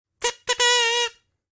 Kazoo Fanfare - Free to Use Sound Effects
Experience the whimsical delight of a kazoo fanfare! This lively sound effect captures the essence of celebration with its playful and slightly nasal tone, reminiscent of a small group of kazoos playing in unison. Perfect for adding a touch of humor and cheer to your projects, this unique fanfare is ideal for comical or celebratory moments in various multimedia endeavors.